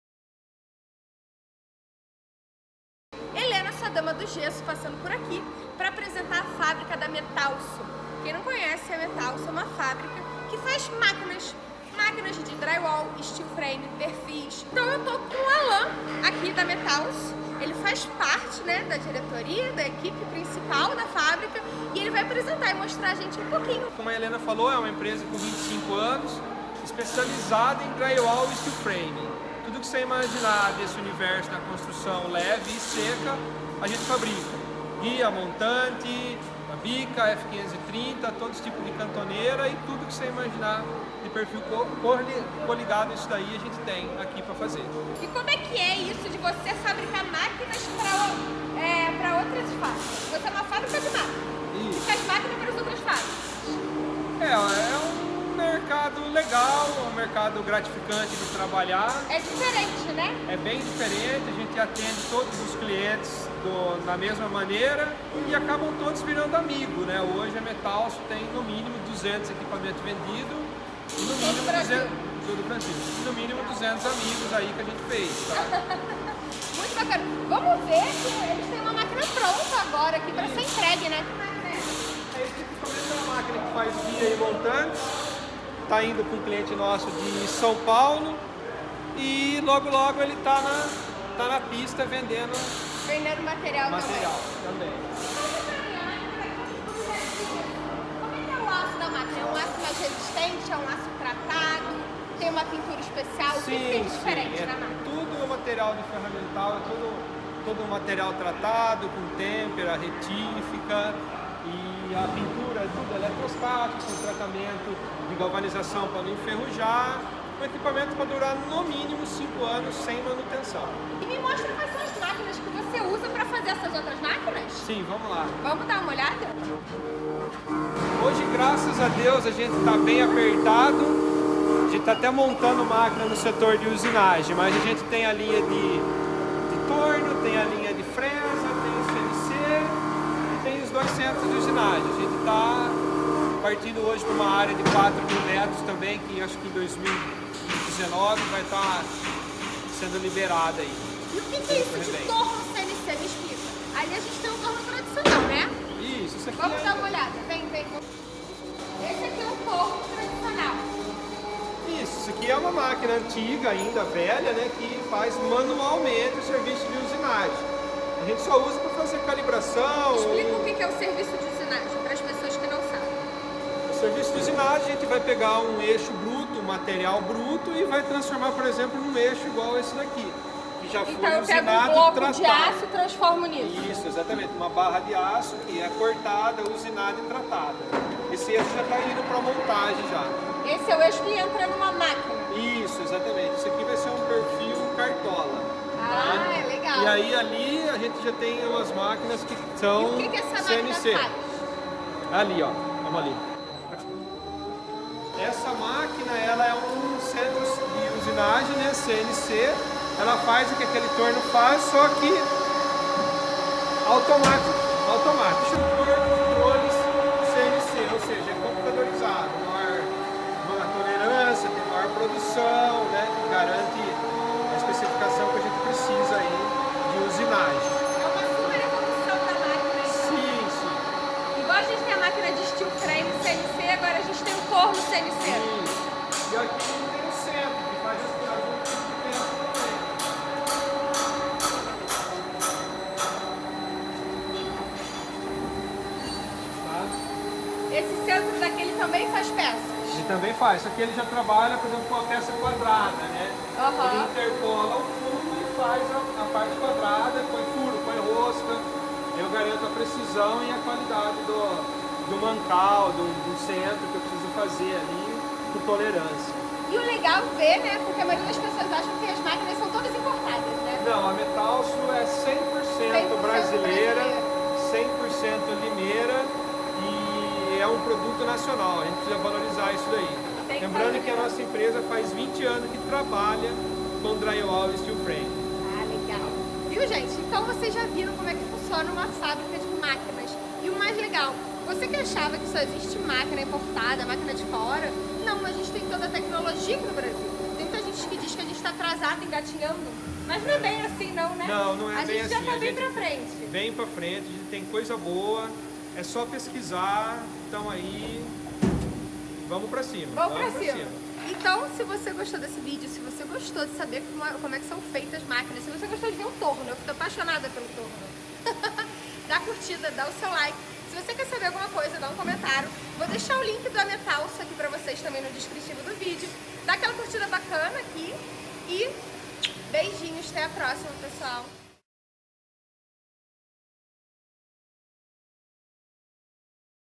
COMO É FEITA UMA MÁQUINA DE STEEL FRAME? | Visita a Metalso
Estou aqui na fábrica da Metalso para mostrar para vocês como são feitas as máquinas de Steel Frame, Drywall e perfis.

Para guiar essa visita, o Alan - que faz parte da Diretoria da Metalso - mostrará um pouquinho sobre como a fábrica funciona na hora de construir máquinas para obras de construção a seco.